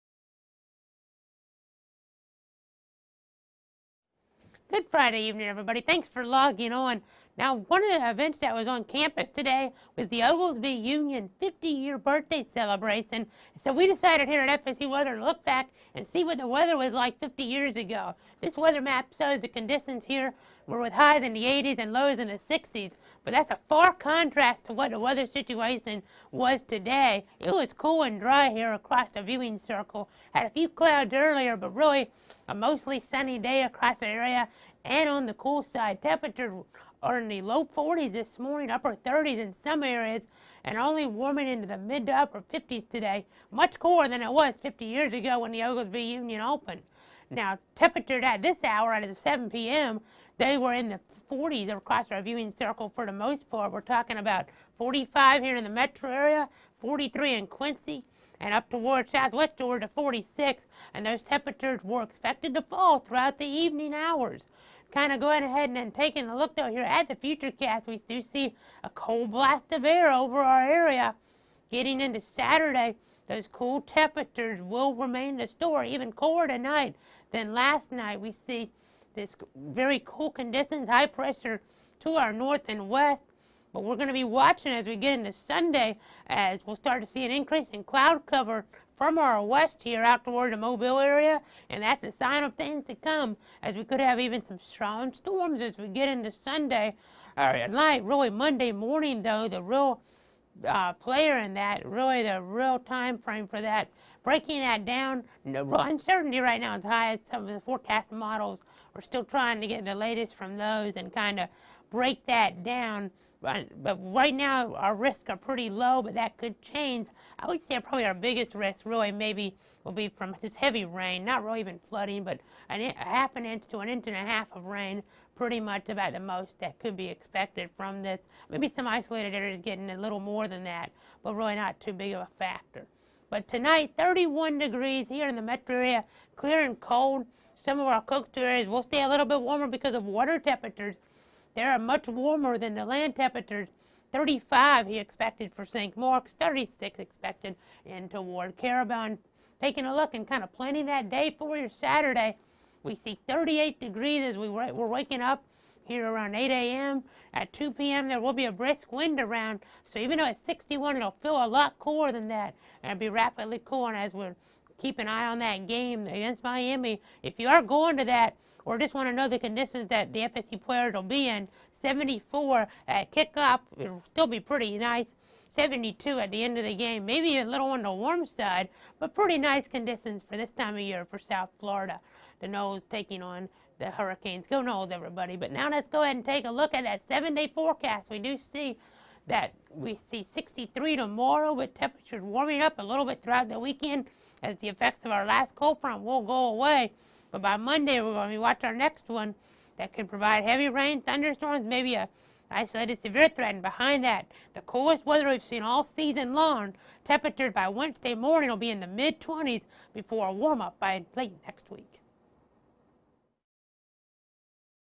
FSU Weather Update: November 14, 2014
BRR! Today was a cold one outside, as highs stayed in the 50s in many areas. We'll warm up a bit before another front moves through on Monday, bringing rain and even cooler weather to the area. How cold?

Casey Cloud tells you just how cold we'll get next week in tonight's FSU Weather Update.